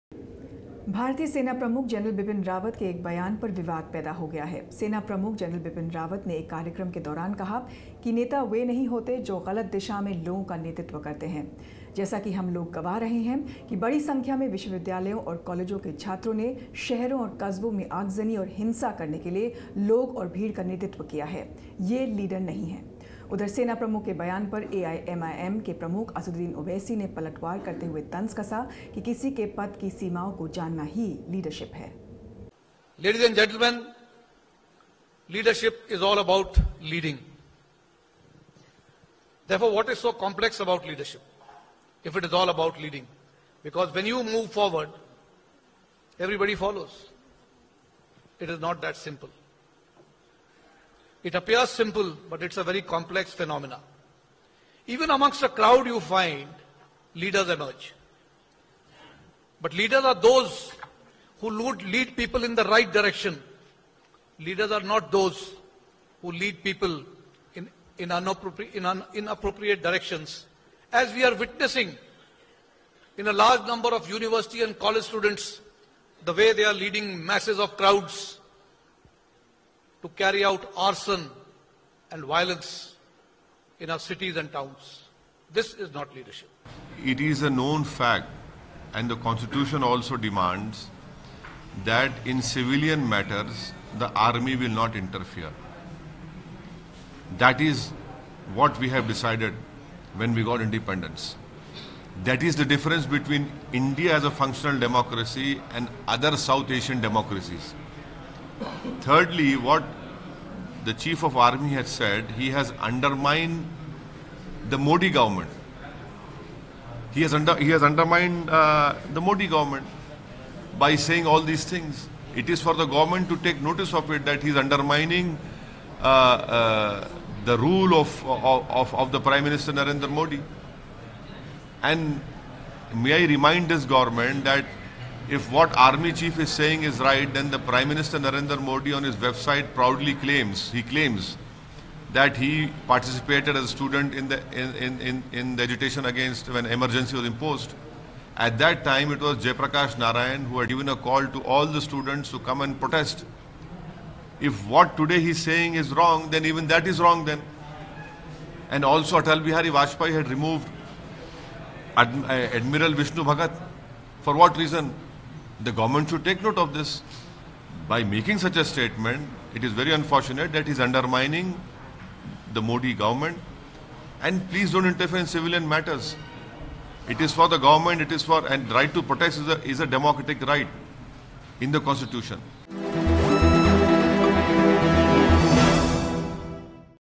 Controversy: Army Chief Hits Out At Anti-CAA Protest Violence
Army Chief General Bipin Rawat courts controversy by speaking out against violence during anti-Citizenship Law protests, says leaders are not those who lead people in arson and violence